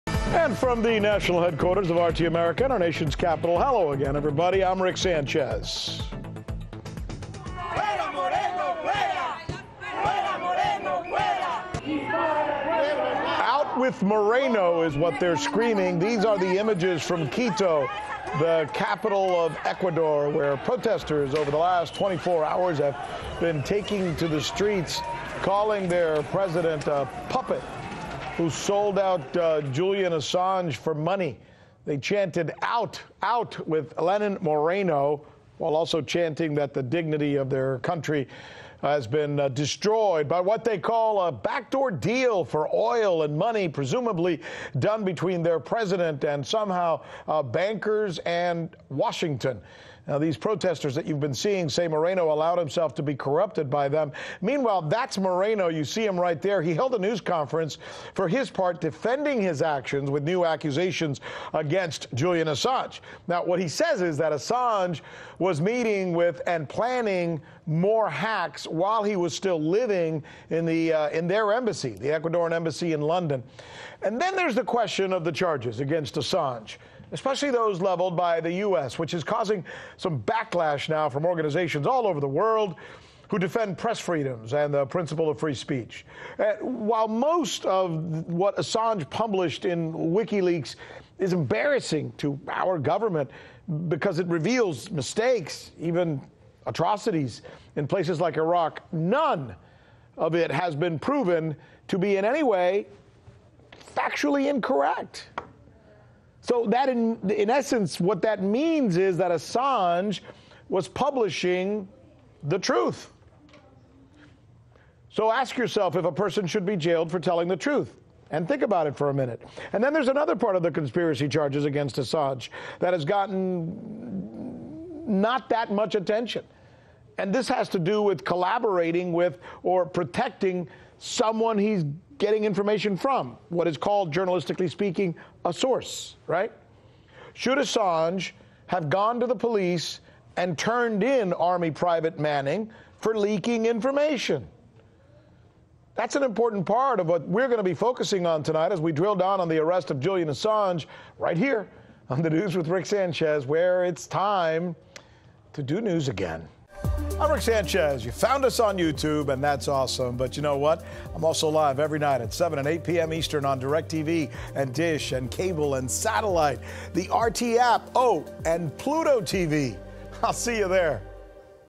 ‘Traitor!’ Riots erupt in Ecuador capital over Assange arrest. (2019)
Rick Sanchez reports on the public outcry in Quito, Ecuador where Ecuadoreans are joining the worldwide chorus of voices protesting WikiLeaks founder Julian Assange’s arrest in the UK. Protestors in Ecuador have come to see President Moreno as “corrupted” by the US and say that he sold out Assange for money.